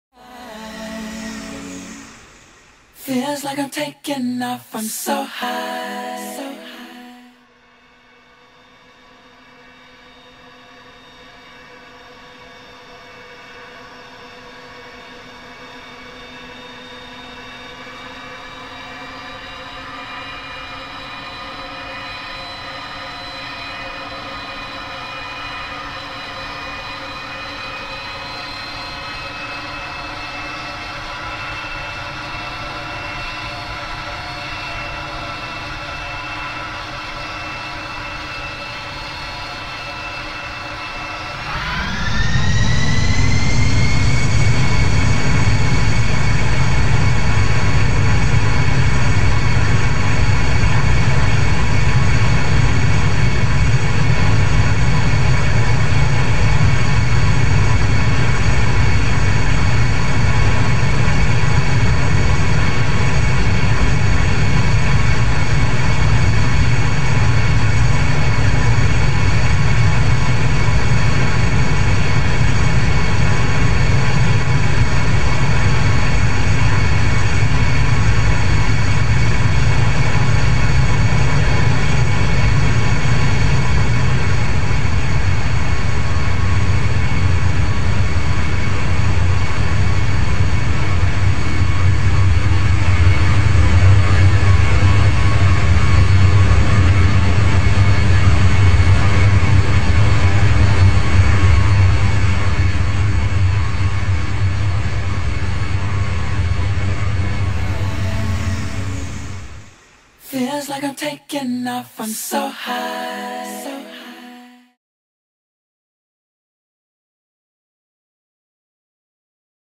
Corsairfly Boeing 747-400 TakeOff.
Wanted to test how good i could take off with this 747-400 i think i did ok tell me if you LIKE .